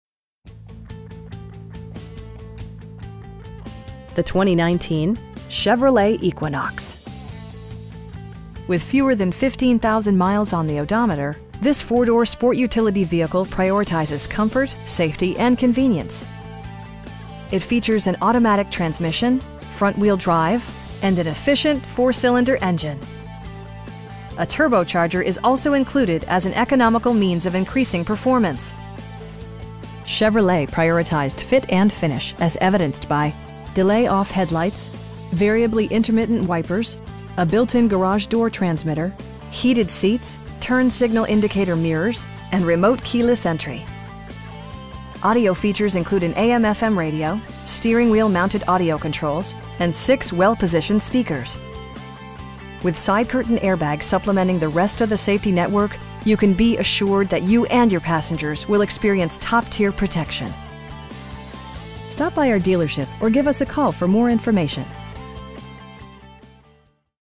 2019 Chevrolet Equinox LT in Tallahassee, FL 32304
Dale Earnhardt Jr. Chevrolet
3127 West Tennessee Street

The 2019 Chevrolet Equinox. With fewer than 15,000 miles on the odometer, this 4 door sport utility vehicle prioritizes comfort, safety and convenience. It features an automatic transmission, front-wheel drive, and an efficient 4 cylinder engine. A turbocharger is also included as an economical means of increasing performance. Chevrolet prioritized fit and finish as evidenced by: delay-off headlights, variably intermittent wipers, a built-in garage door transmitter, heated seats, turn signal indicator mirrors, an overhead console, and remote keyless entry. Audio features include an AM/FM radio, steering wheel mounted audio controls, and 6 well positioned speakers. With side curtain airbags supplementing the rest of the safety network, you can be assured that you and your passengers will experience top-tier protection. We pride ourselves on providing excellent customer service. Stop by our dealership or give us a call for more information.